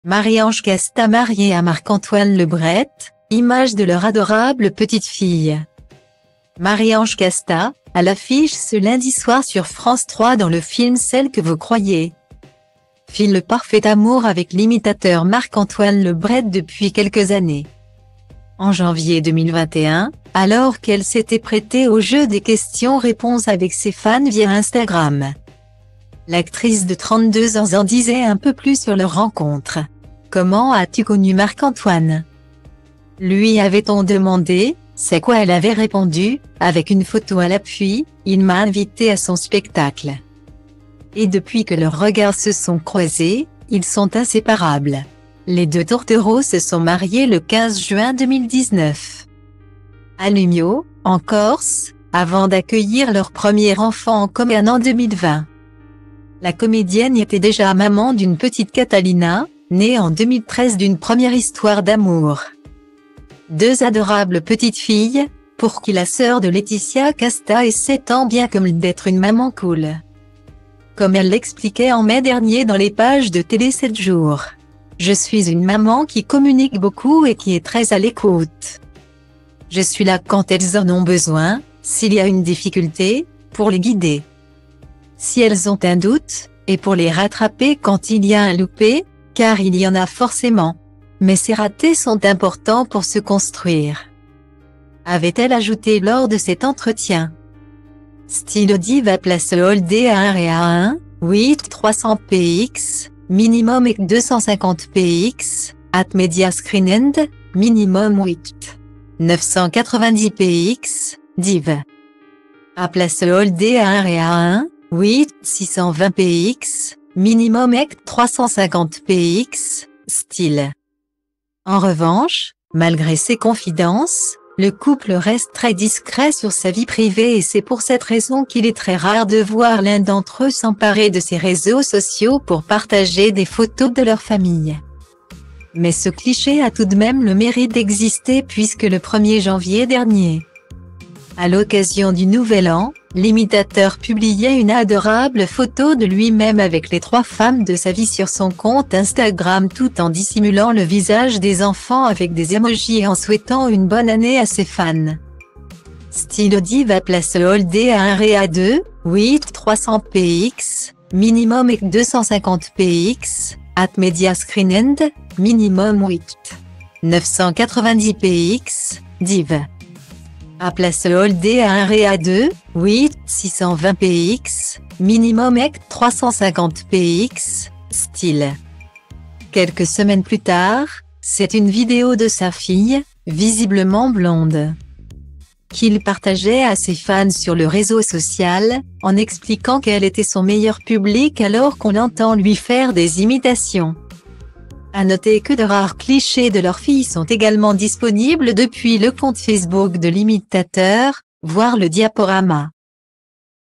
Marie-Ange Casta mariée à Marc-Antoine Le Bret : images de leur adorable petite fille
''Marie-Ange Casta, à l'affiche ce lundi soir sur France 3 dans le film Celle que vous croyez,file le parfait amour avec l'imitateur Marc-Antoine Le Bret depuis quelques années.En janvier 2021, alors qu'elle s'était prêtée au jeu des questions-réponses avec ses fans via Instagram,thank for watching my video your sub my channel